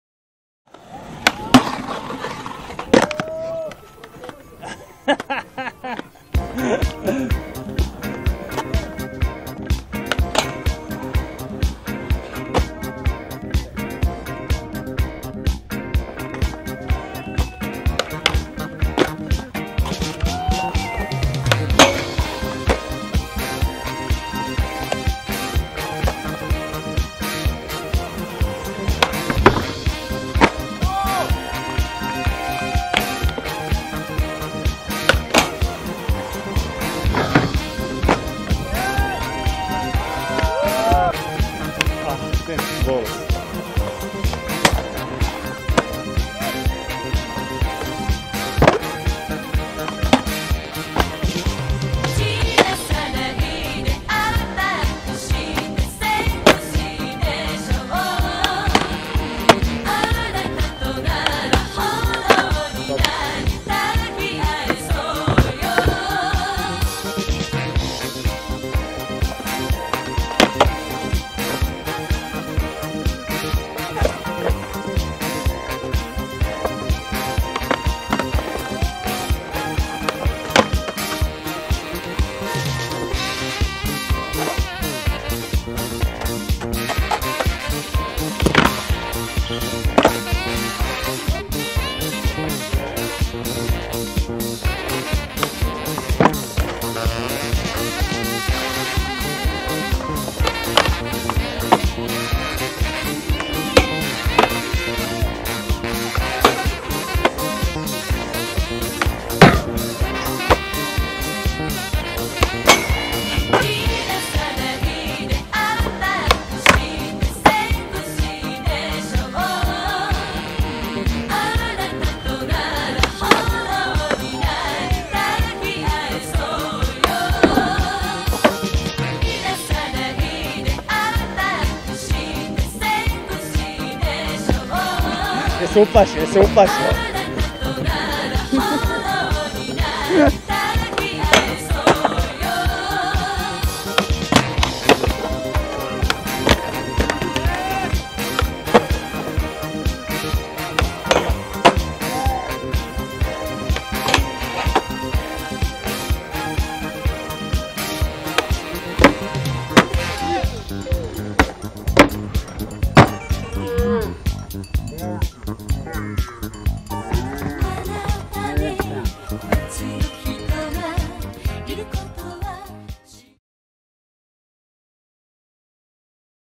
Park funk 6
Sunshine Easter session 2022.

Skaters:
Ernest Mazarekić
Vjekoslav Banović
Tomislav Vranić
Moreno Mijatović
Marino Mazarekić
Petar Spiedlick

Camera:
Paro / Vrana

Edit:
Tica

Sunshine crew  2022.